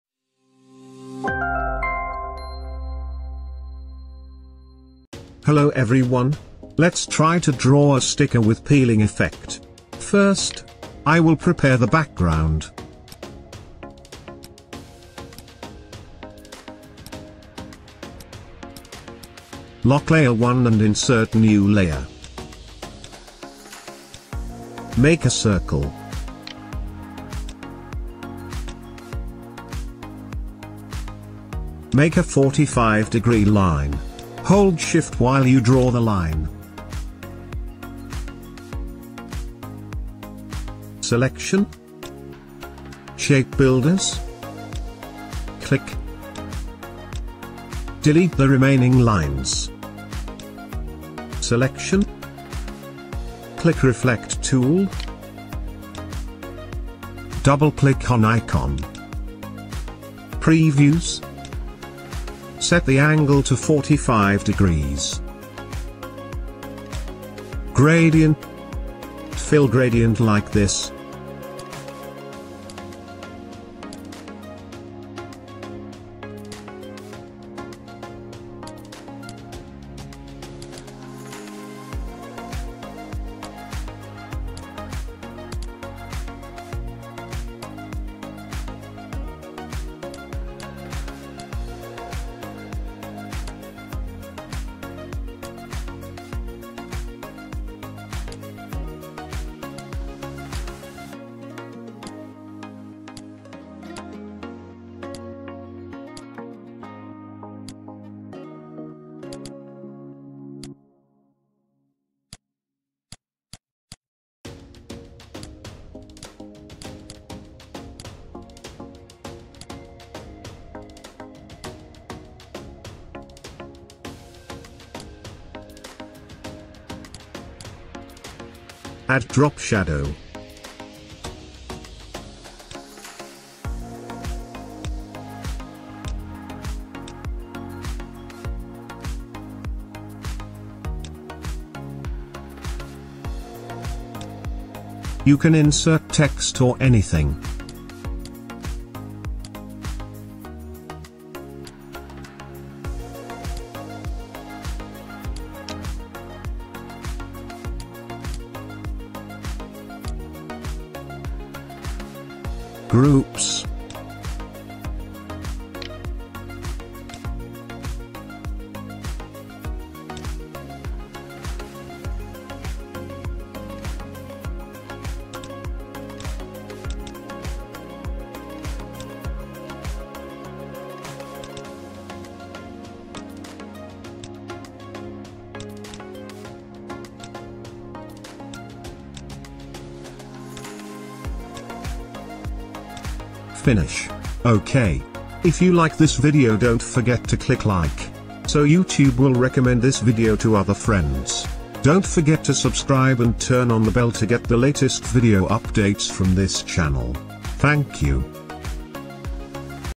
How to Make Peeling Effect stickers - Adobe Illustrator Tutorial
One way to make stickers more attractive is the peeling effect. you can try it.